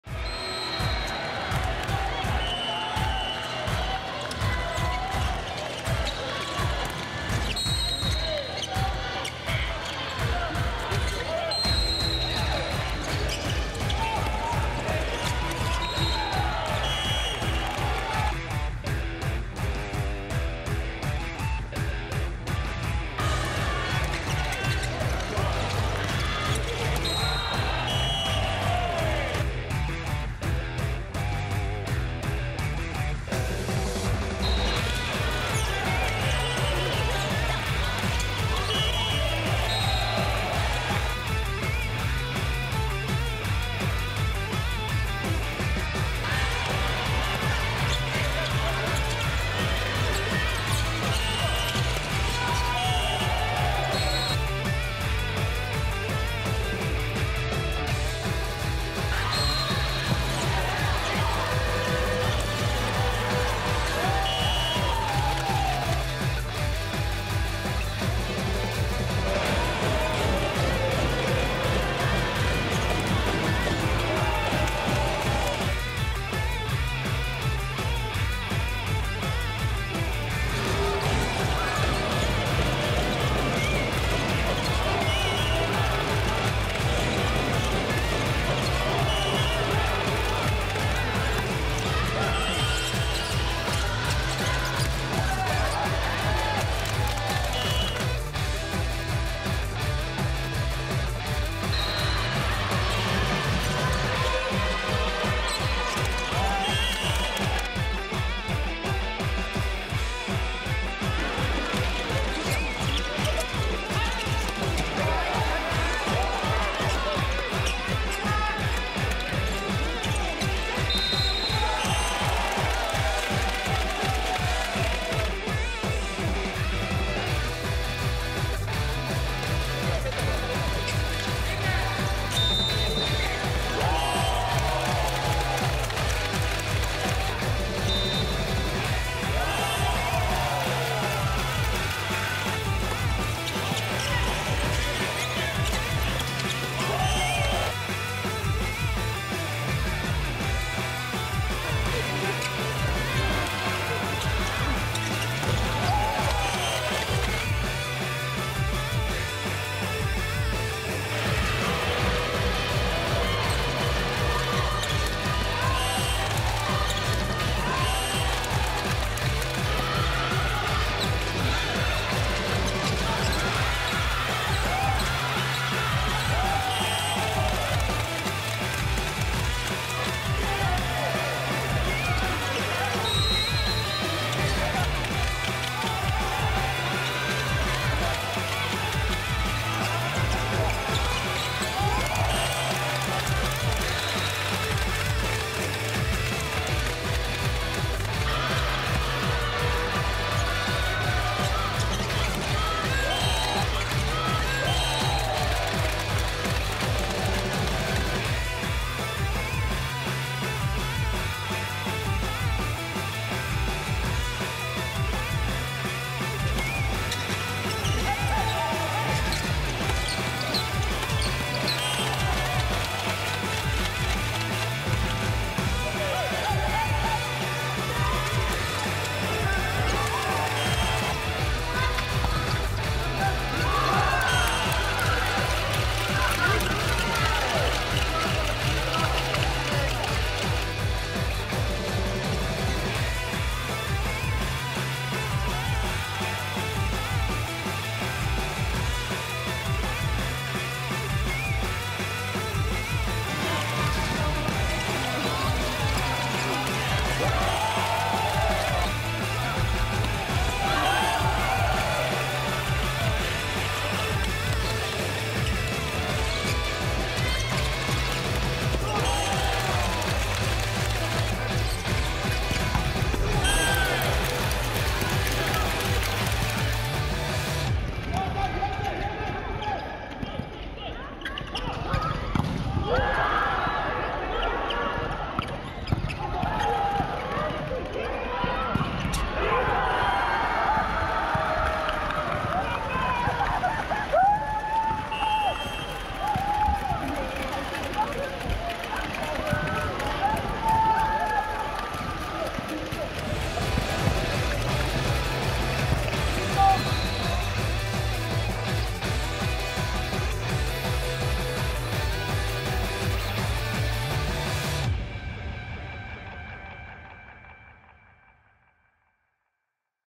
【ツーセッター】セッターの得点集
第65回近畿6人制バレーボール大会においてツーセッターで最後まで戦い抜き優勝したパンサーズ。
セッターの得点集を公開！！

↓大会のフル動画はコチラ↓
〇大阪府バレーボール協会 チャンネル